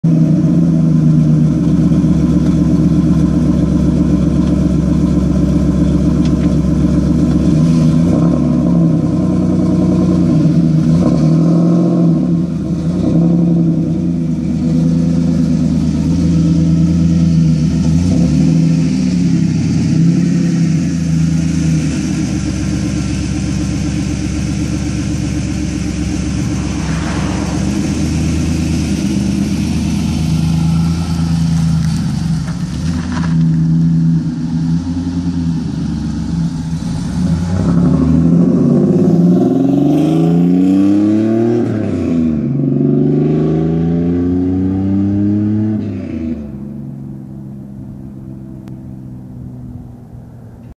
Cammed 1997 Camaro RS drive away
Compcams 255 / 262 .517 / .498 lift mild / aggressive camshaft. Pacesetter short-tube headers and Y-pipe w / cat-less exhaust to a Flowmaster 40 dual outlet with some other mods.